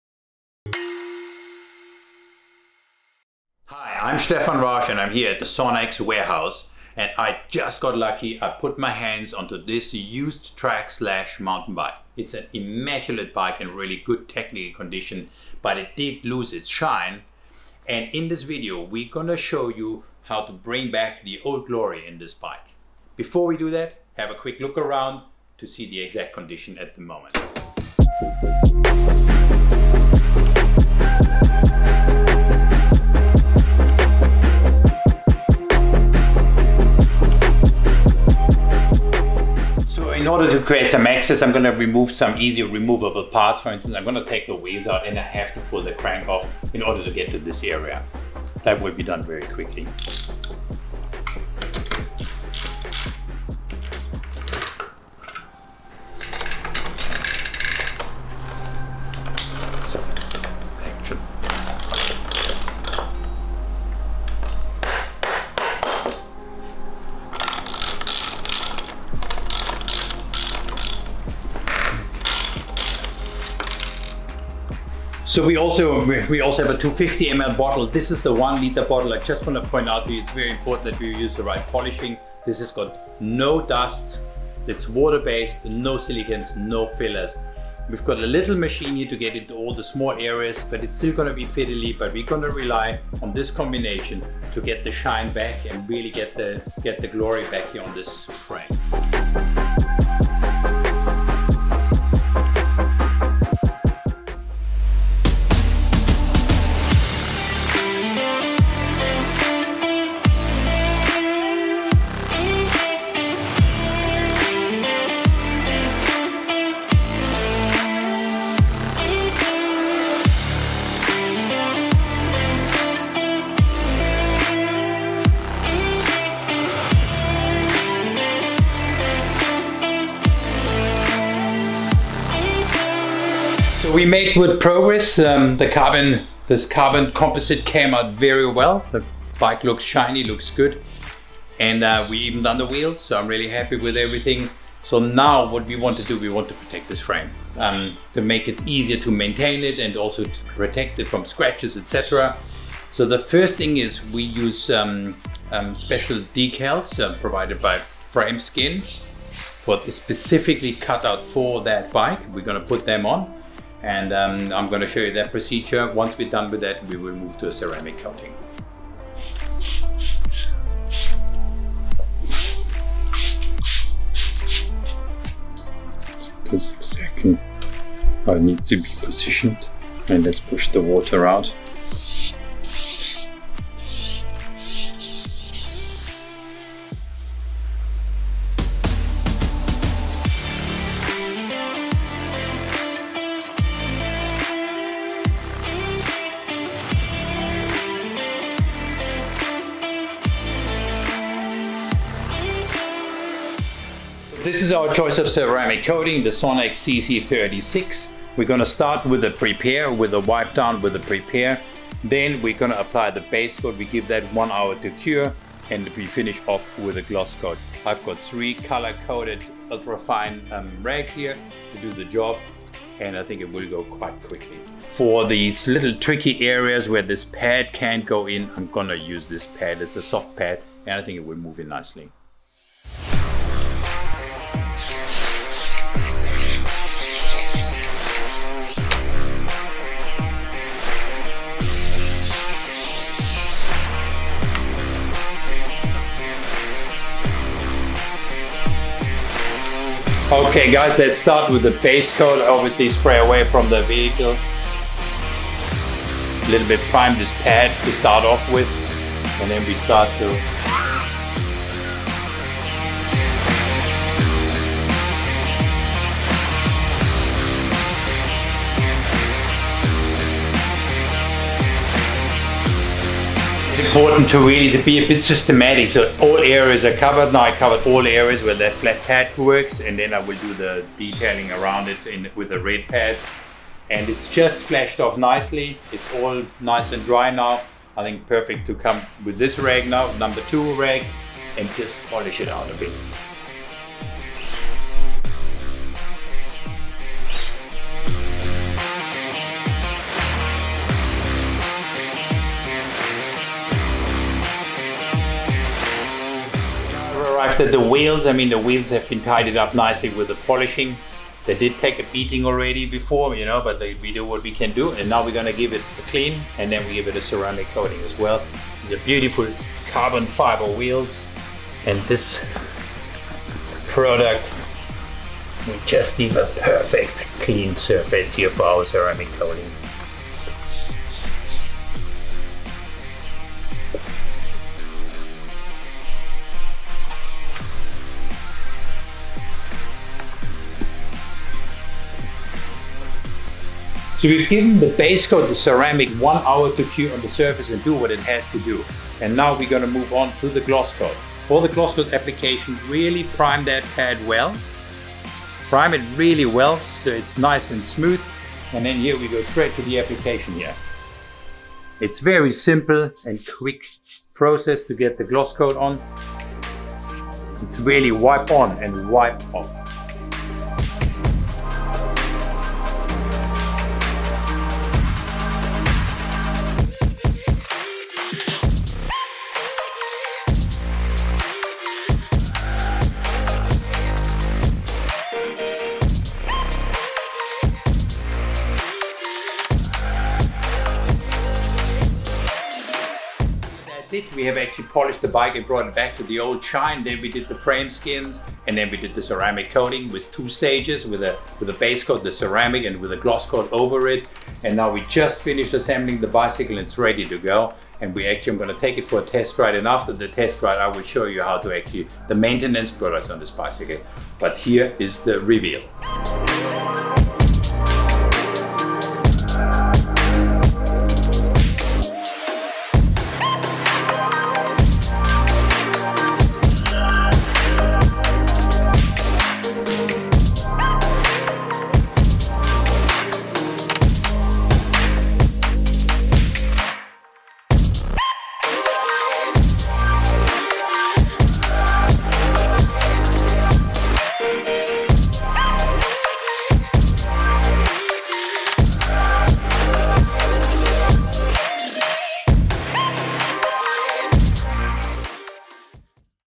Detailing + Ceramic Coating a Mountain Bike! | SONAX Australia
In this video we go over how we polish & ceramic coat a mountain bike!

We also added a Frame Skin to this bike, for maximum protection from the Australian elements. The SONAX team loves some MTB action, so we hope this video helps you to maintain your bike or restore a used one.

For any more information on the products we used or to purchase them